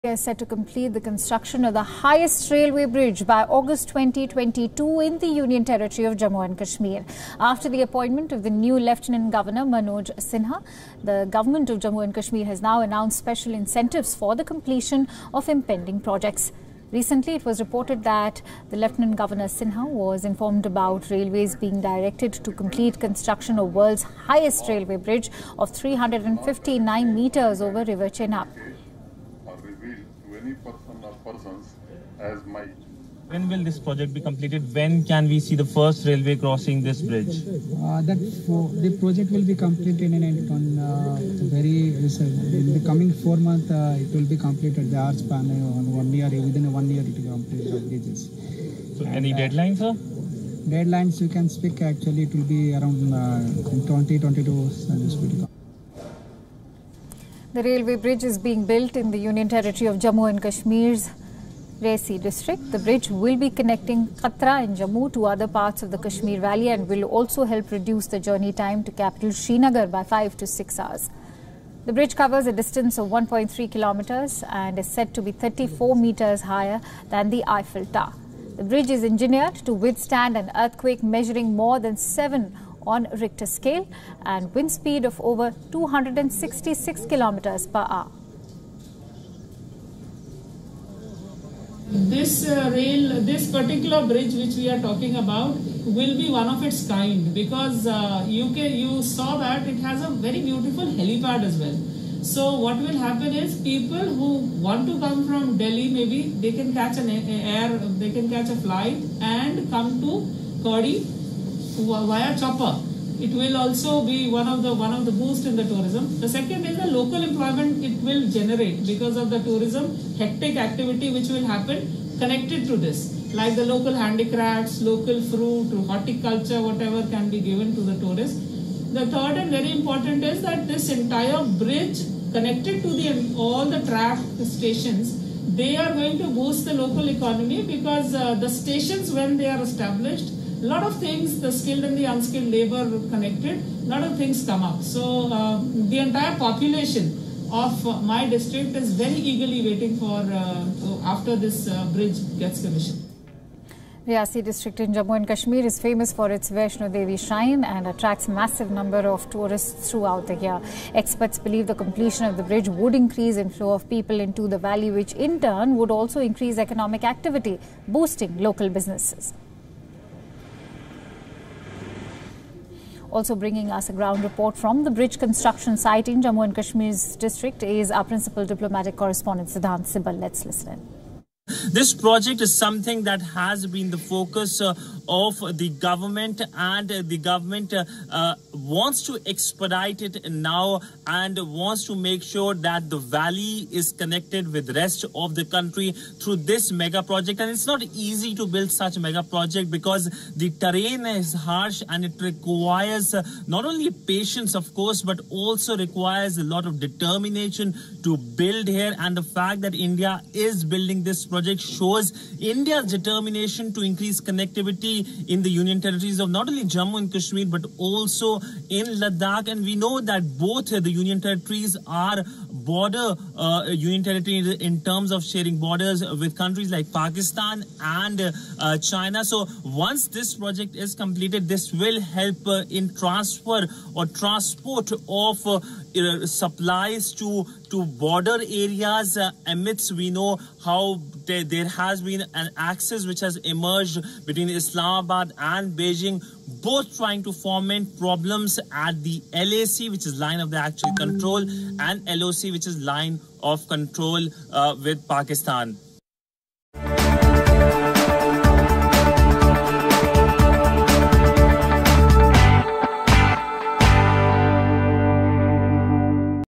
1.3 Km long railway bridge with height of 359 metres | World's highest railway bridge | J&K
1.3 Km long railway bridge with height of 359 metres | World's highest railway bridge | J&K

Union territory of Jammu & Kashmir's Reasi district will soon host the world's highest railway bridge